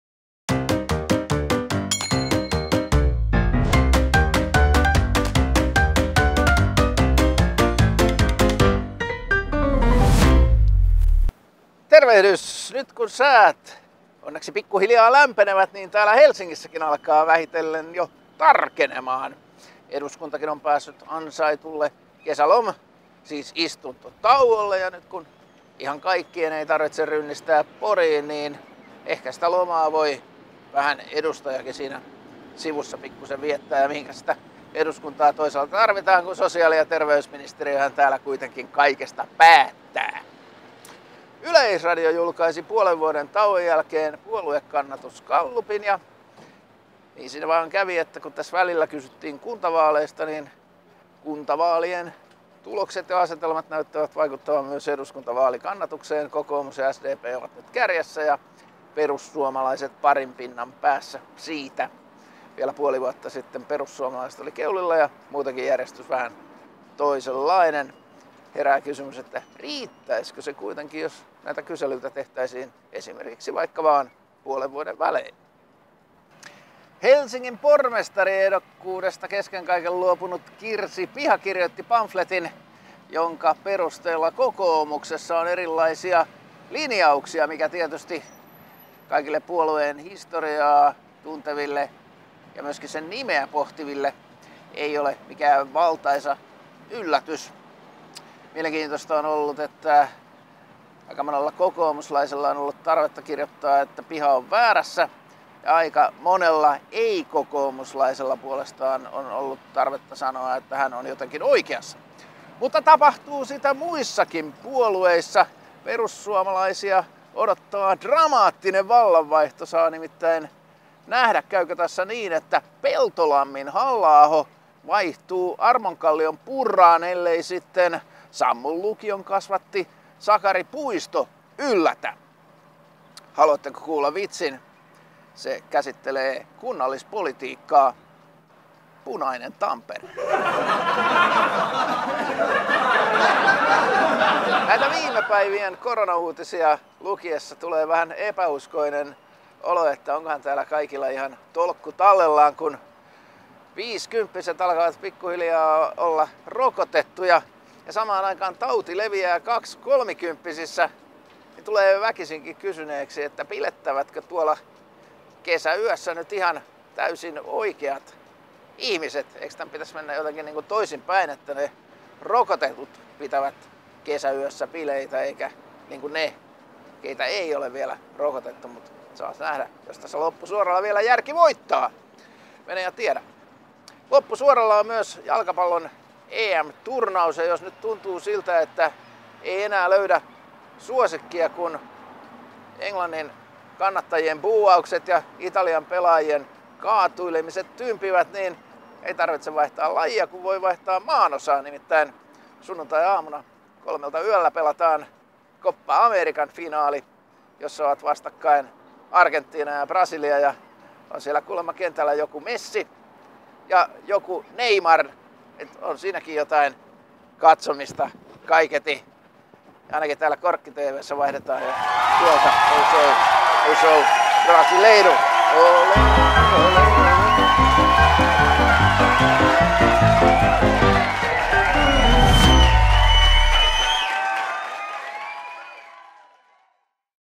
KorkkiTV: Kirsi Piha väittää, että kokoomuksessa on erilaisia linjoja
Puolueen nimen perusteella moisen ei pitäisi olla kenellekään valtava yllätys.